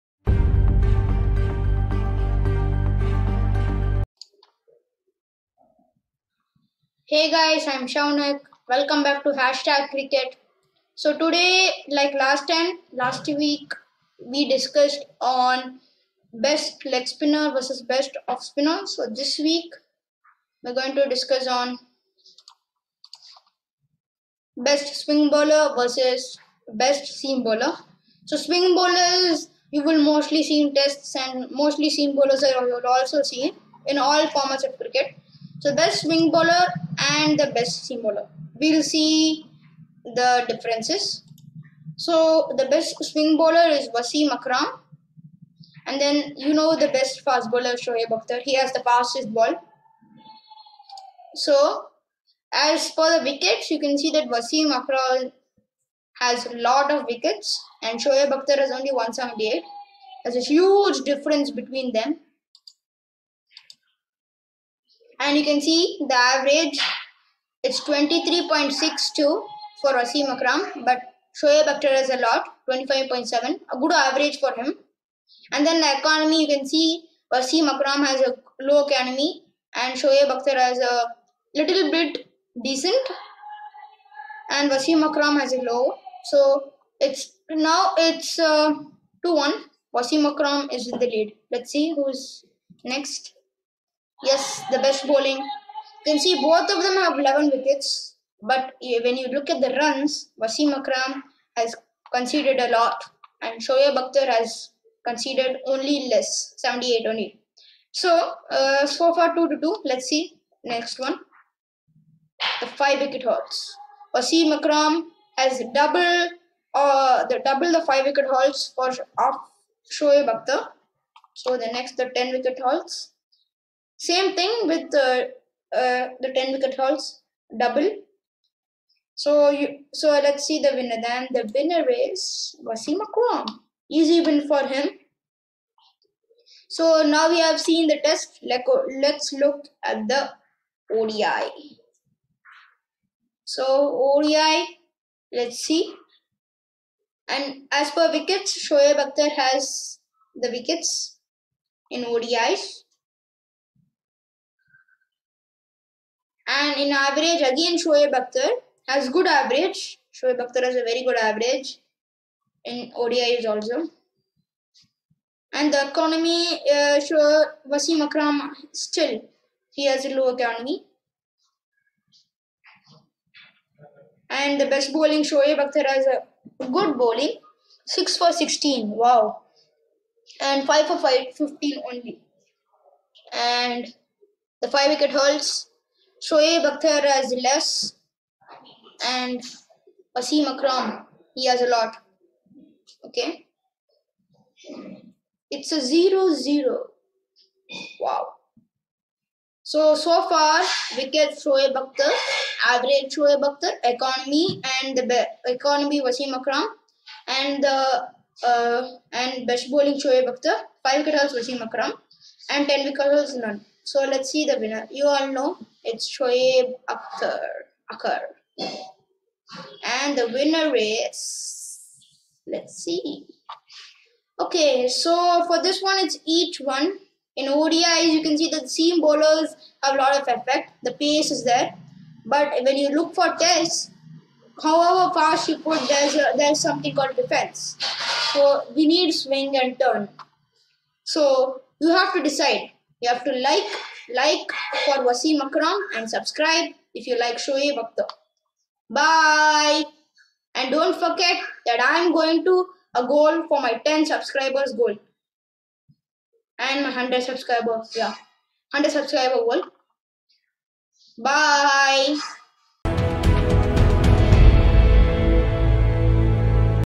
PACE BOWLING VS SWING BOWLING
TWO BOWLERS FACE TO SEE WHO IS THE WINNER
COMMENT THE BOWLERS